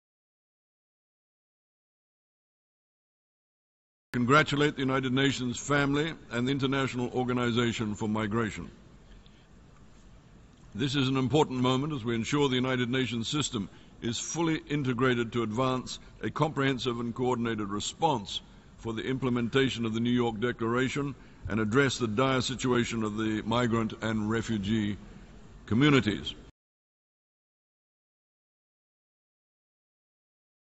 Na ONU, líderes adotam documento em defesa de migrantes e refugiados
Líderes mundiais adotaram nesta semana a Declaração de Nova York, em defesa dos direitos de migrantes e refugiados. Os Estados-membros se comprometem a iniciar negociações que levem a uma conferência internacional e à adoção de um pacto global para uma migração segura, ordenada e regular em 2018.

O presidente da 71a sessão da Assembleia Geral da ONU, Peter Thomson, elogiou a decisão. “Este é um momento importante, pois asseguramos que o Sistema ONU está completamente integrado a uma resposta a uma resposta avançada, abrangente e coordenada para a implementação da Declaração de Nova York e a abordagem da situação dramática das comunidades de migrantes e refugiados”, disse. Os detalhes